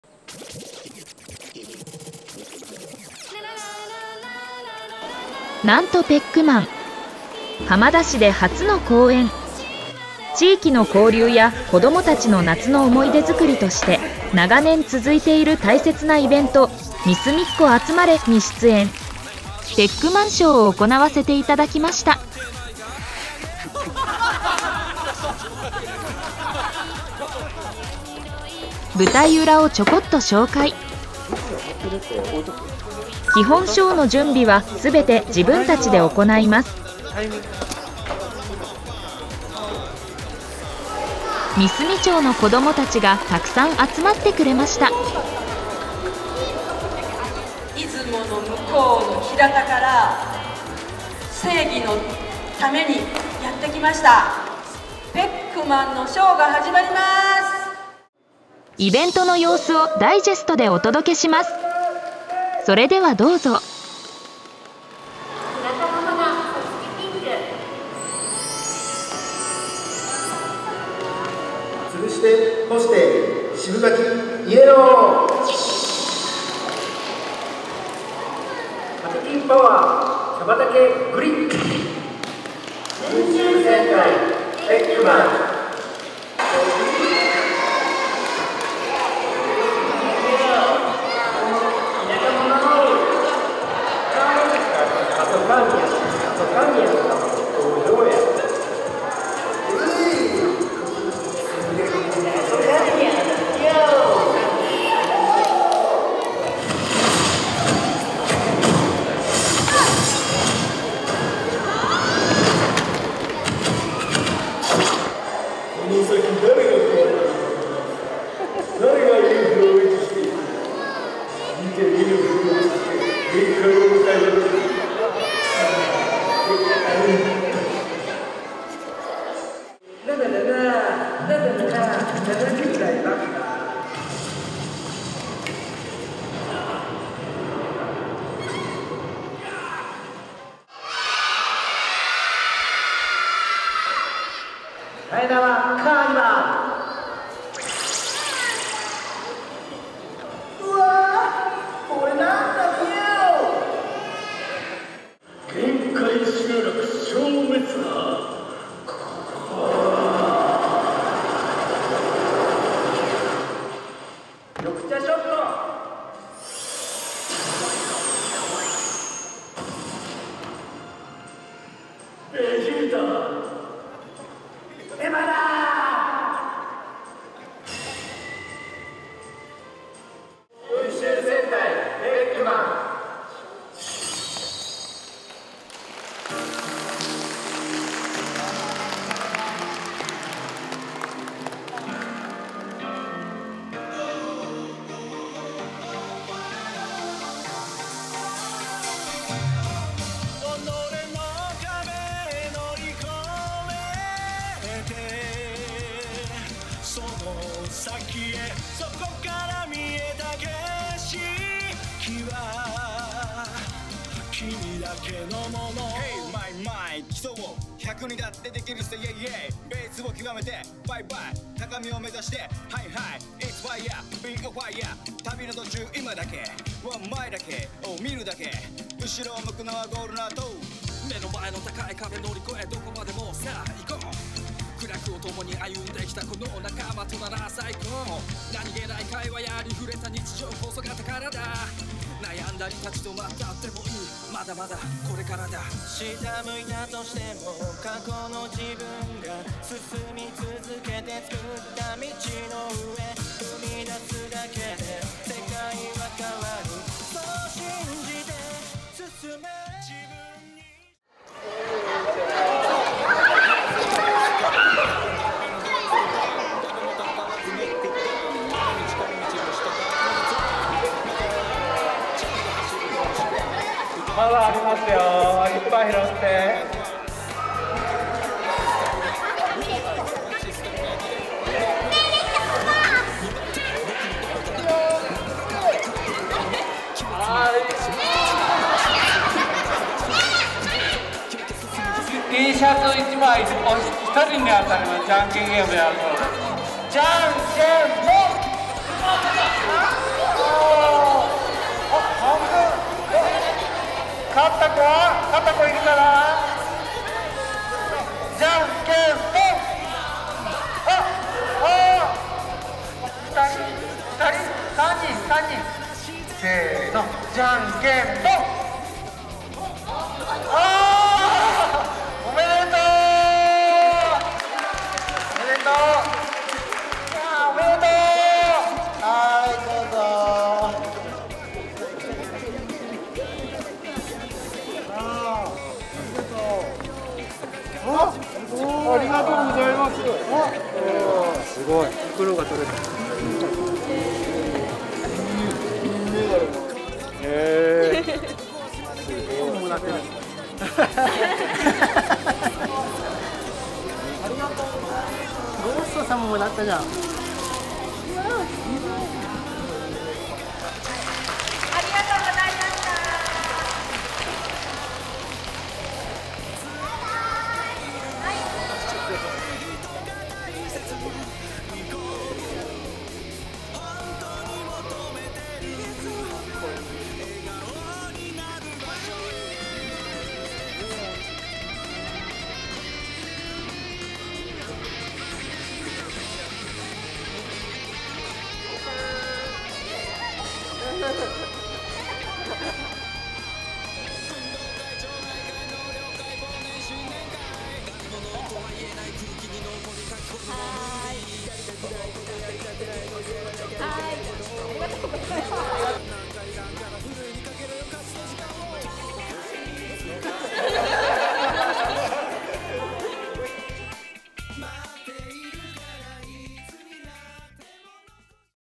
【初浜田公演】みすみっ子集まれ！ペックマンショー
2024年8月3日に浜田市三隅町、三隅まちづくりセンターで行われたペックマンショーです！浜田のみんな！三隅のみんなありがとう！オリンピックにちなんで金メダルのプレゼントも貰いました！感謝感激ペックマーン☆ヒーローショー